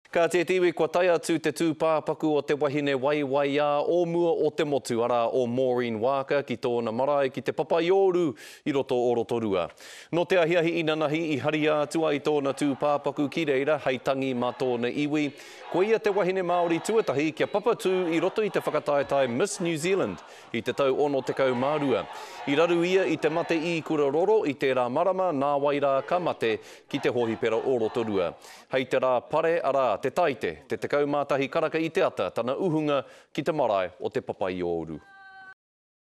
Maureen Waaka returns home
Former beauty queen Maureen Waaka is now at her home marae of Te Papaiouru, Rotorua. She was taken there yesterday afternoon. She was one of the first Māori women to win the Miss NZ crown in 1962.